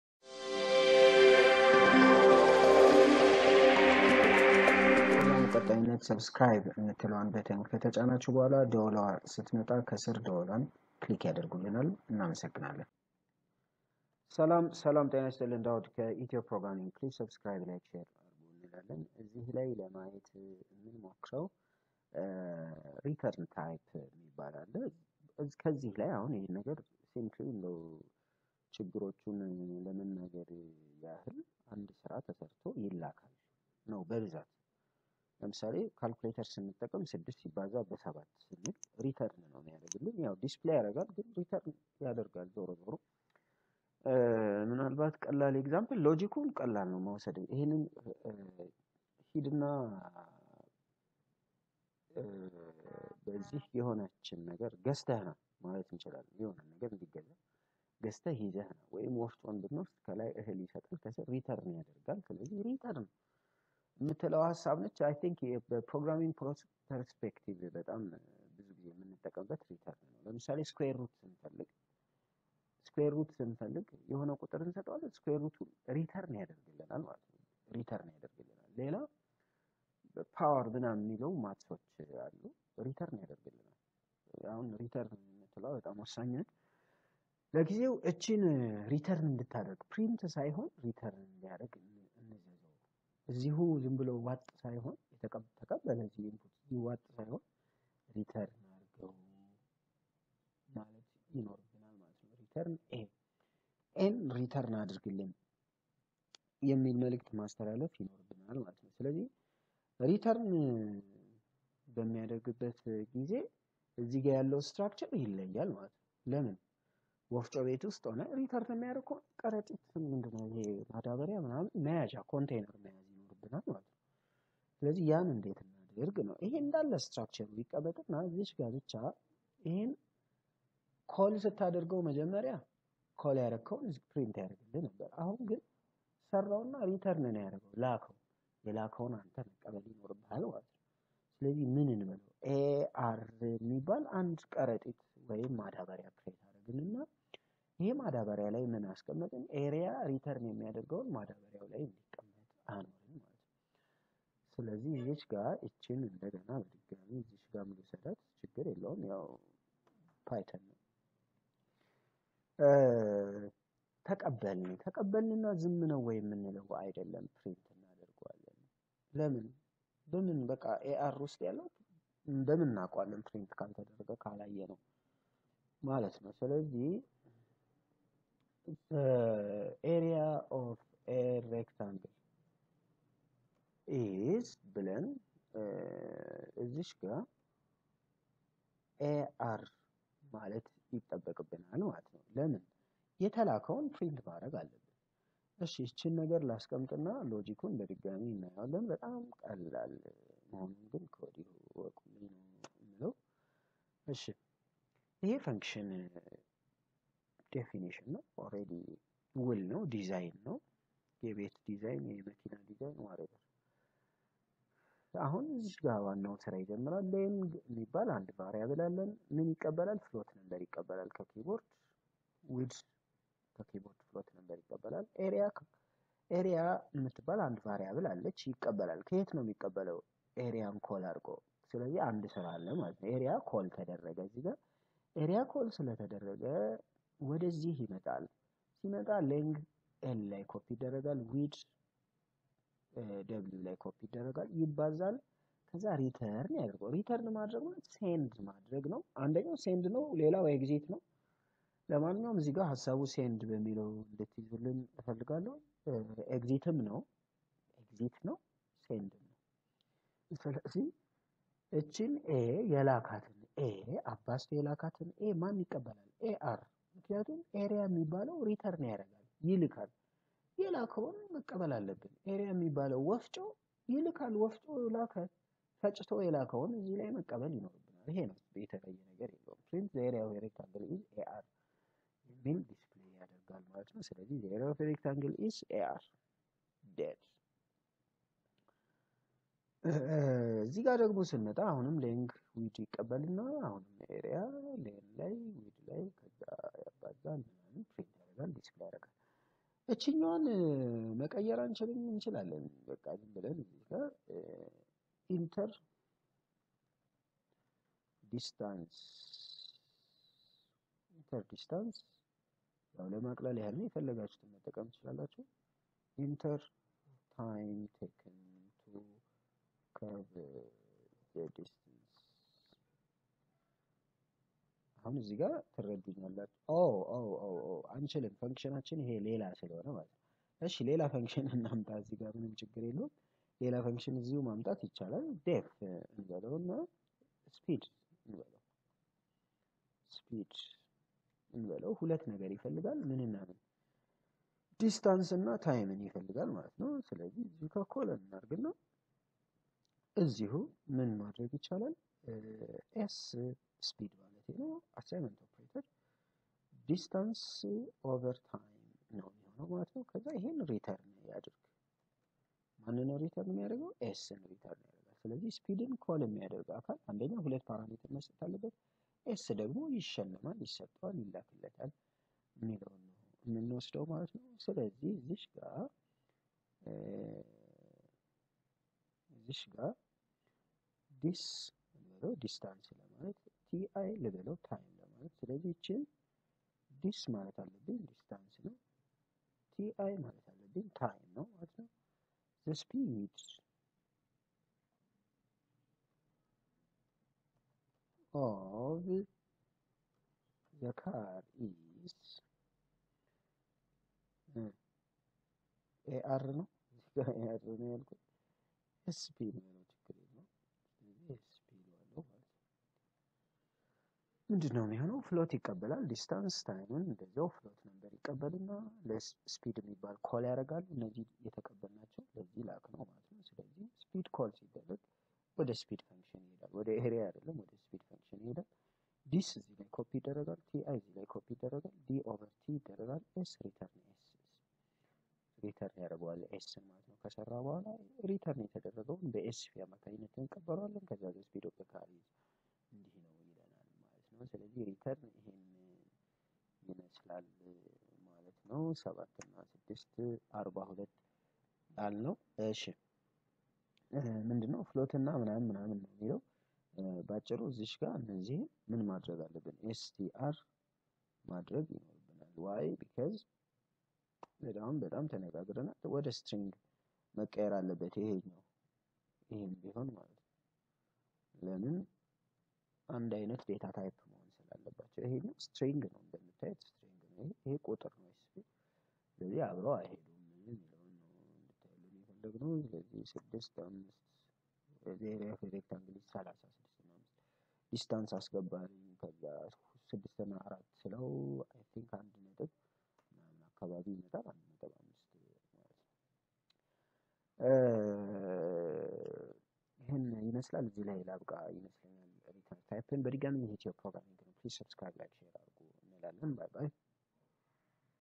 Lecture 29: Python Functions Return Type Programming Tutorial in Amharic | በአማርኛ
The videos are tutorials regarding the Python Programming language prepared for Amharic speakers all over the world.